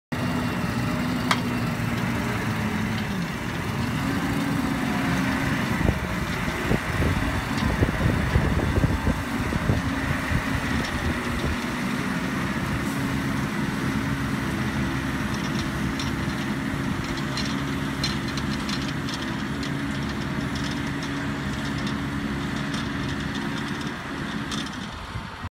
Lemken Jewel 8 M plough
The Lemken Jewel 8 M plough was on show in Hungary